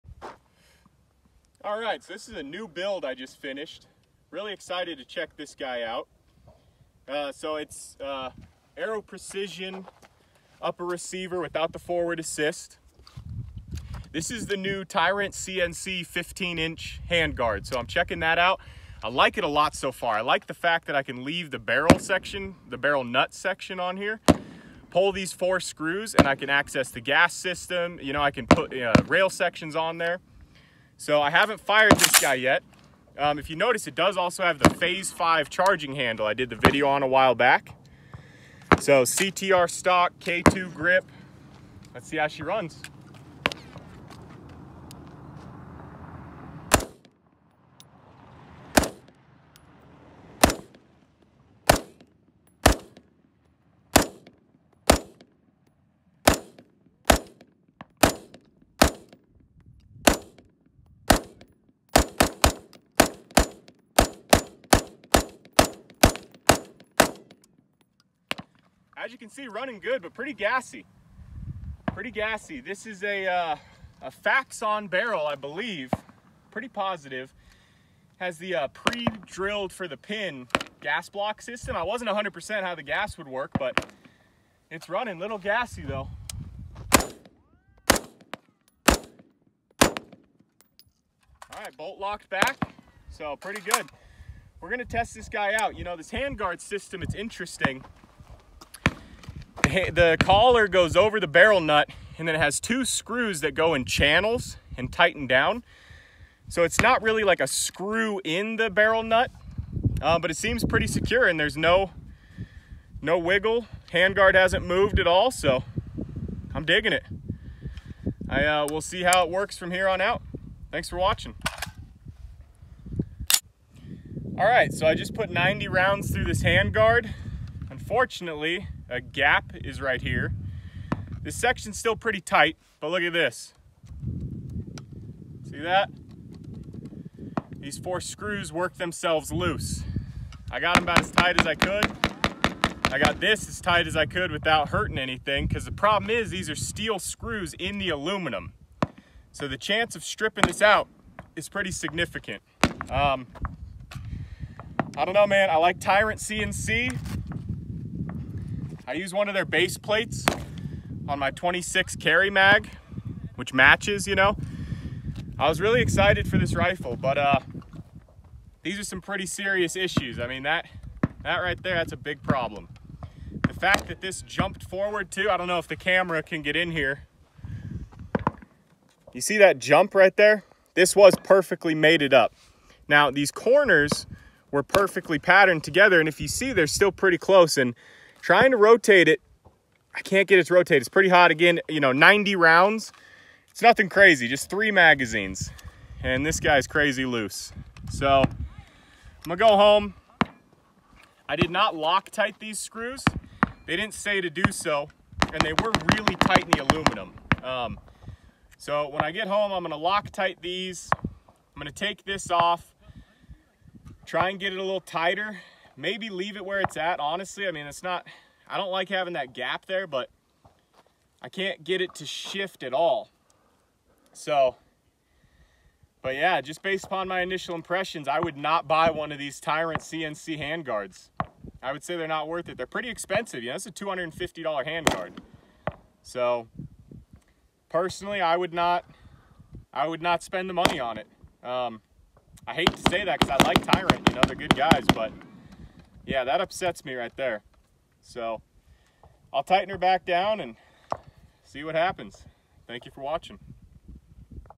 Mandalorian AR15 Build with Tyrant CNC Handguard (Fail).
This is one of, if not THE, coolest rifles I’ve ever built or shot. It should have been my favorite rifle. Unfortunately the failures and quality issues of the Tyrant CNC handguard make this rifle near useless.

I have spoke with the guys at Tyrant and I gave them details on how to improve this handgaurd: I truly hope they do as they are a great company and this product has the potential to be incredible.

I will post a follow up video trying the rifle with the barrel section tightened down absolutely as tight as it will go and the other screws loctited.

I have more videos on the way: new and unique AR15 builds, unique suppressor hosts, classic pistols and more.

God Bless. Tell your partner you love them.